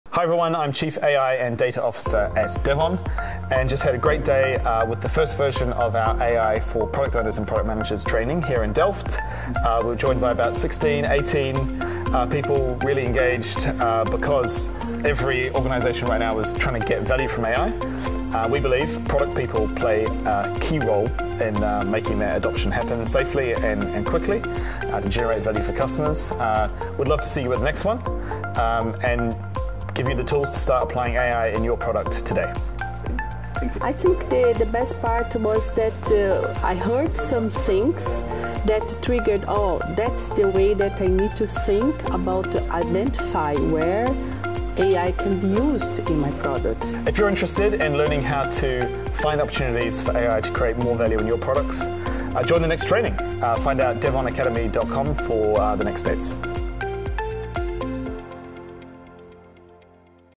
AI for Product Owners & Managers - Course Insights & Attendee Success Stories
AI for Product Owners & Managers training is packed with real-world applications, cutting-edge AI integration techniques, and the full potential of AI in your projects. Hear firsthand from our latest success story - a testimonial from a recent attendee who has seen remarkable career growth and product success by applying the knowledge and skills gained.